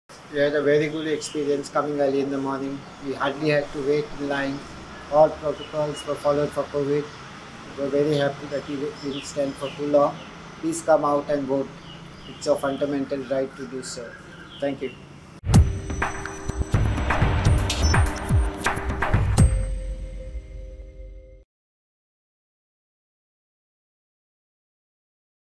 Voters at Assembly Constituency - 30 Fatorda, Ravindra Bhavan
Voters from AC- 30 Fatorda, Ravindra Bhavan talk about their their wonderful experience voting today.